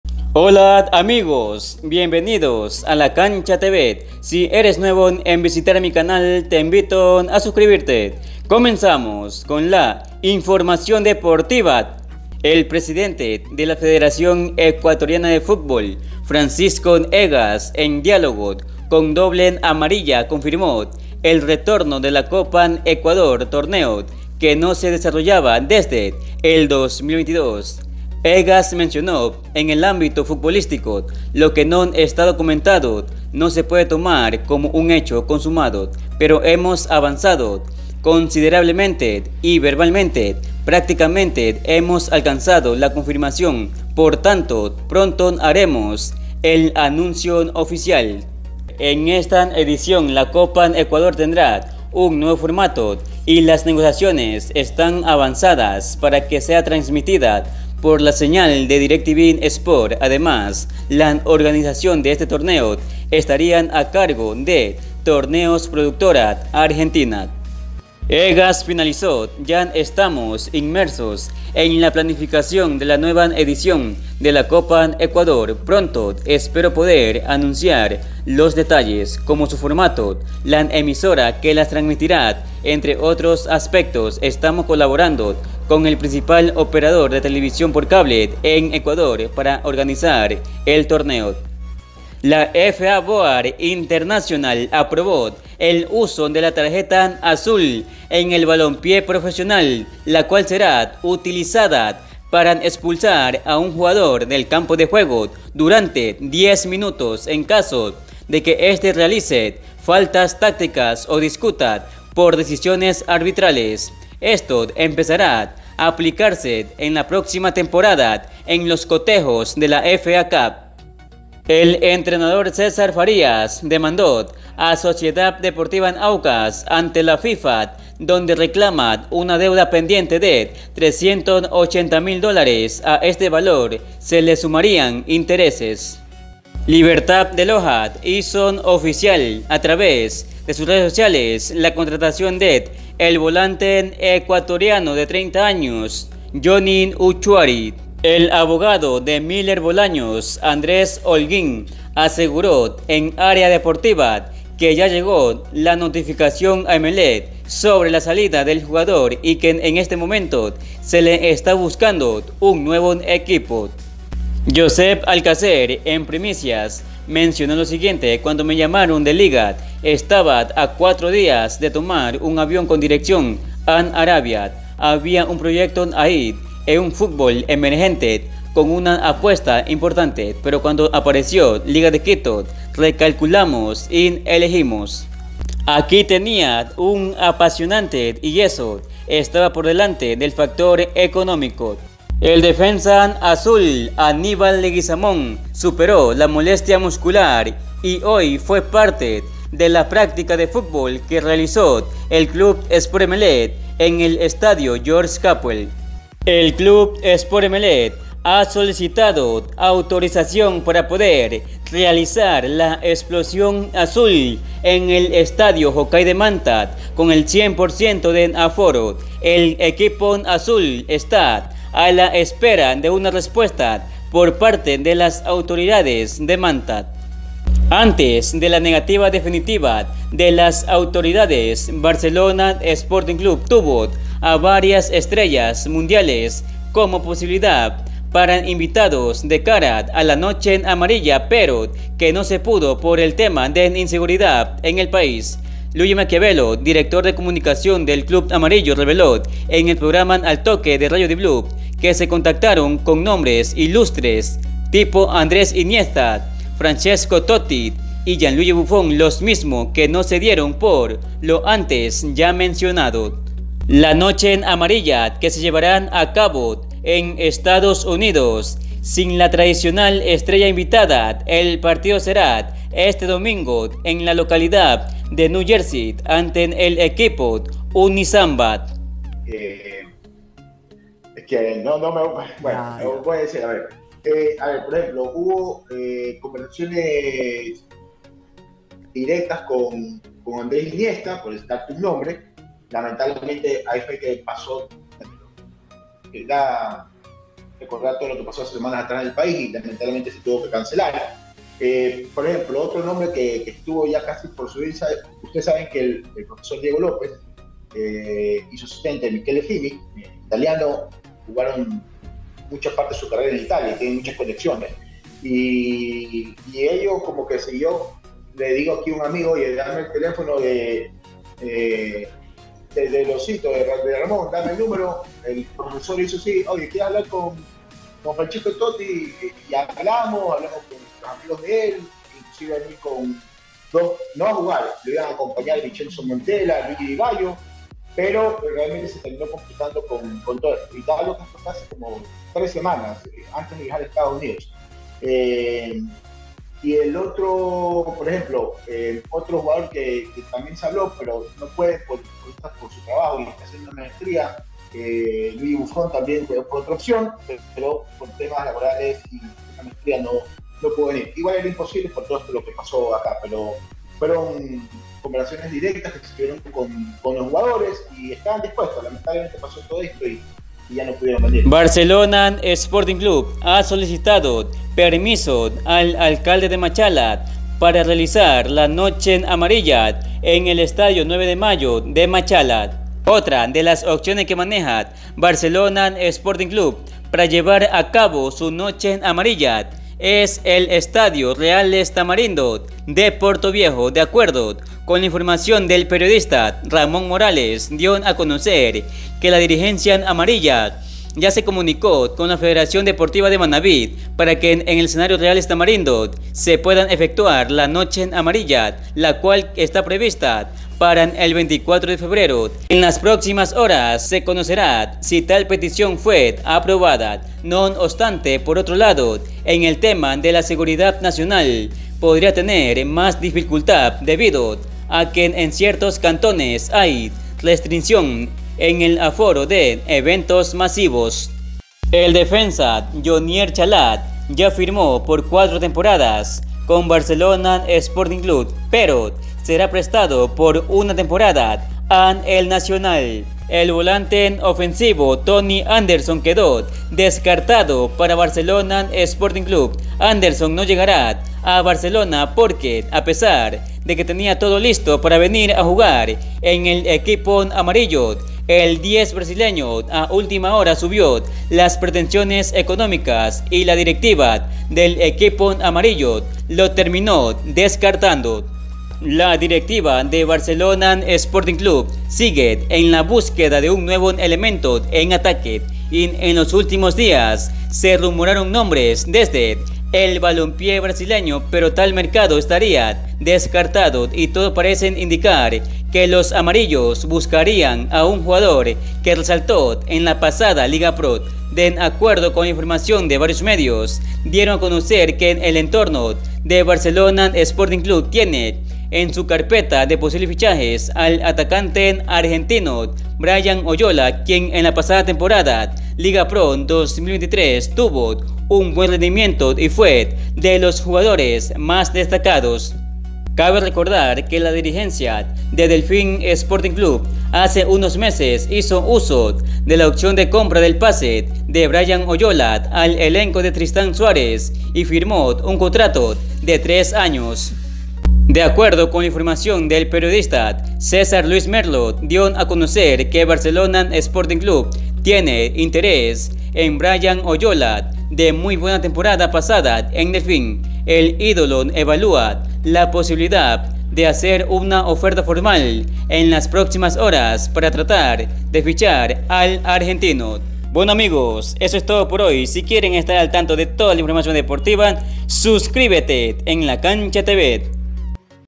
B.OYOLA JUGARÁ EN BSC|T.ANDERSON NO VIENE A BSC|NOCHE AMARILLA EN PORTOVIEJO EXPLOSION AZUL EN MANTA
BRIAN OYOLA JUGARÁ EN BSC?|THONNY ANDERSON NO JUGARA EN BSC|NOCHE AMARILLA EN PORTOVIEJO Y  EXPLOSION AZUL EN MANTA Y MAS NOVEDADES DE BSC,ESTRELLAS INVITADA A LA NOCHE ANARILLA ,EMELEC,LDU,AUCAS,COPA ECUADOR Y MAS si quieren estar al tanto de toda la INFORMACIÓN DEPORTIVA SUSCRIBETE EN LA CANCHA TV